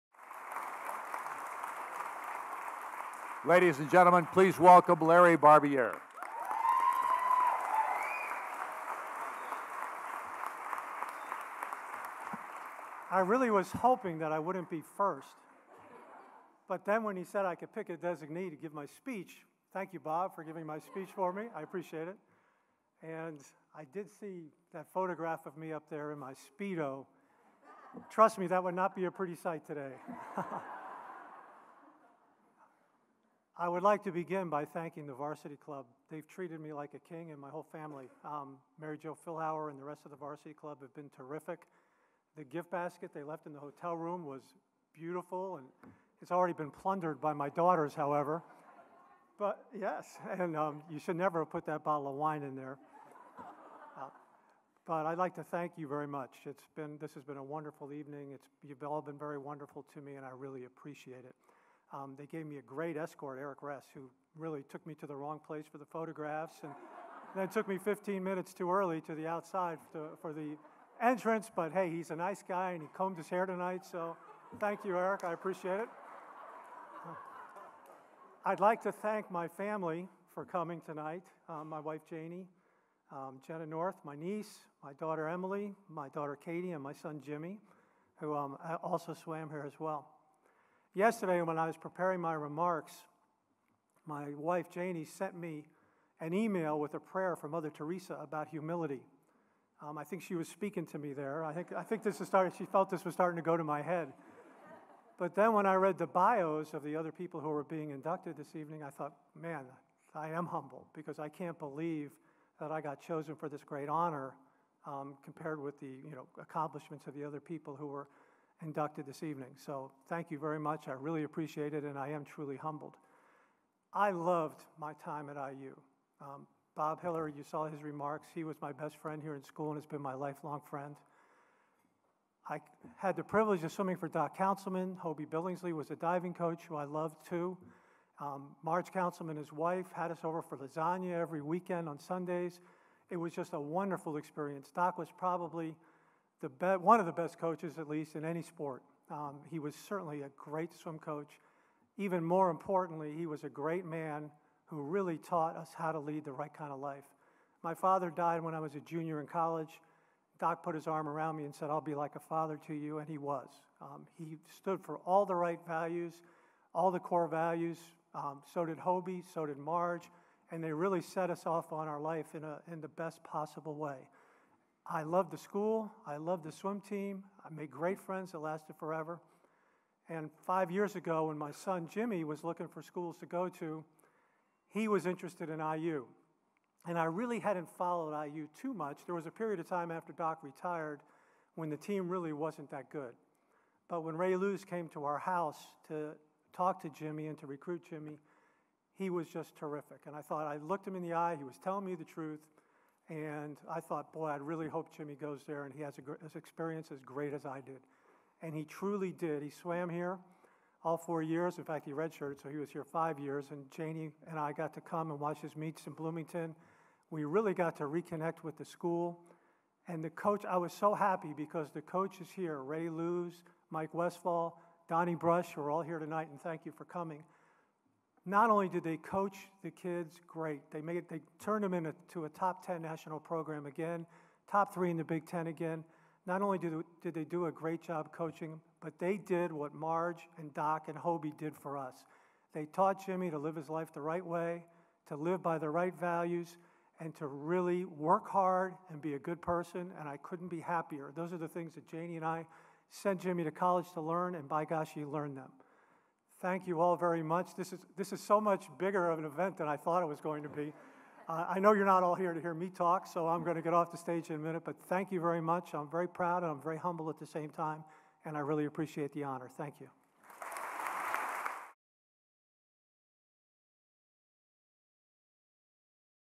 2013 Indiana Athletics Hall of Fame: Larry Barbiere
Larry Barbiere (Swimming - 1970-73) - One of the key figures in a golden era of IU swimming, Barbiere won swimming letters in 1970, 1971, 1972 and 1973. During those years he was part of four Big Ten and NCAA Championship teams. Barbiere is an 11 time All-American, and was the NCAA champion in the backstroke in 1970. Barbiere won Big Ten titles in the 100 and 200 backstrokes and 200 individual medley and was on the 400-medley relay championship team in 1970. He was a member of 1968 U.S. Olympic Team where he placed 4th in the 100 backstroke.